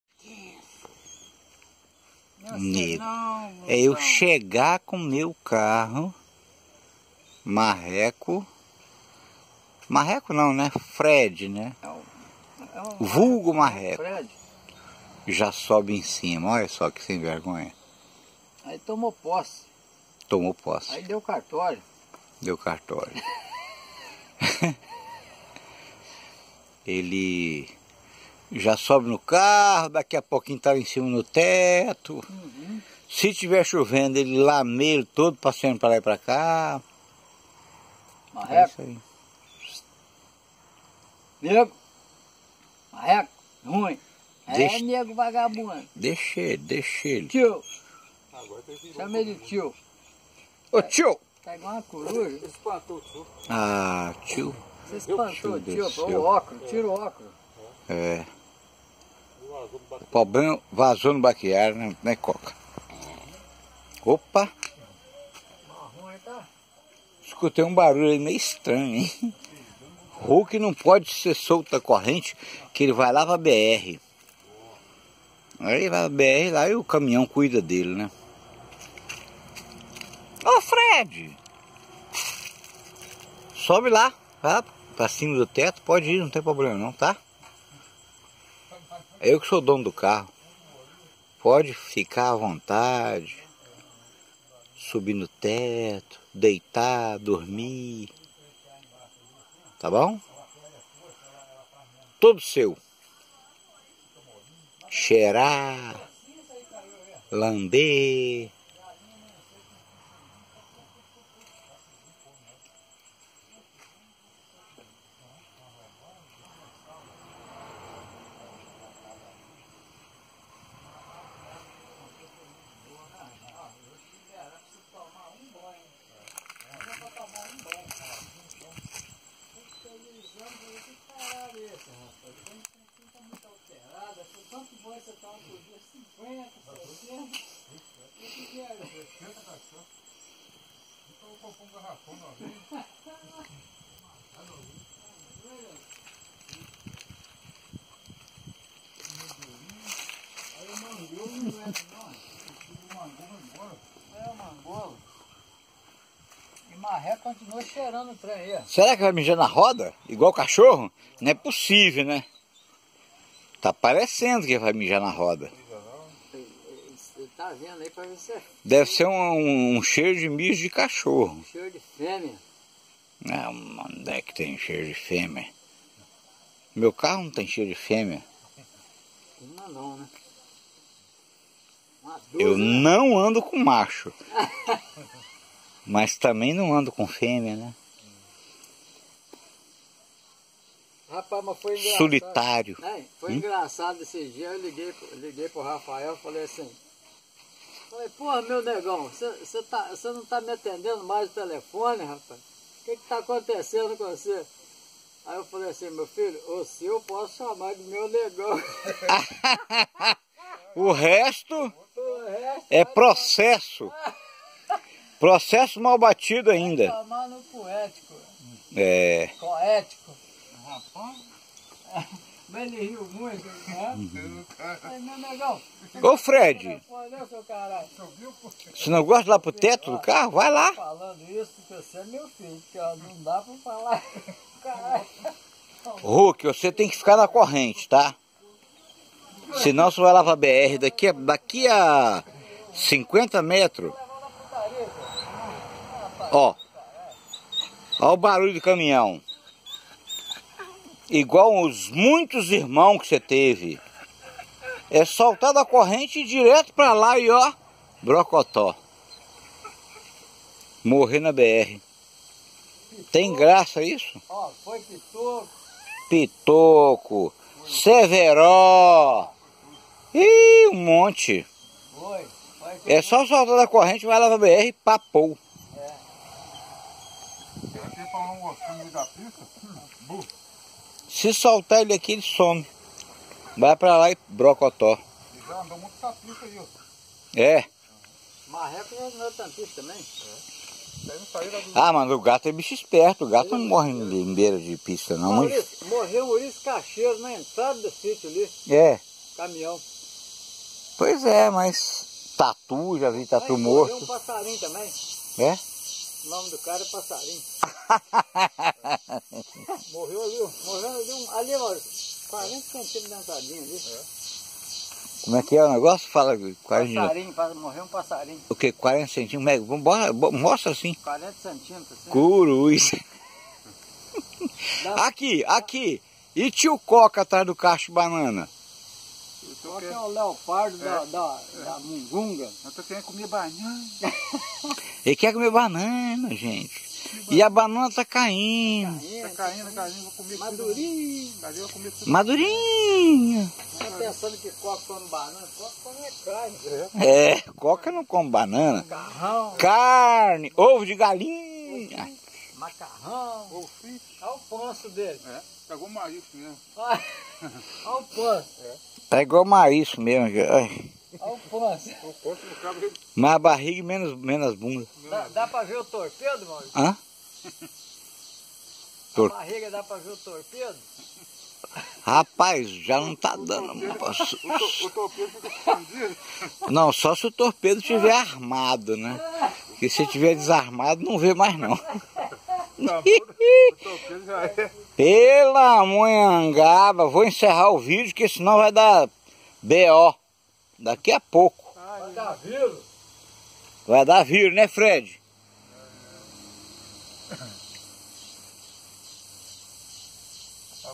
Fred já toma posse do meu carro. Hulk não pode ser solto que vai pra BR morrer...
Fred já toma posse do meu carro logo na minha chegada de Vitória.Hulk não pode ser solto que vai pra BR morrer...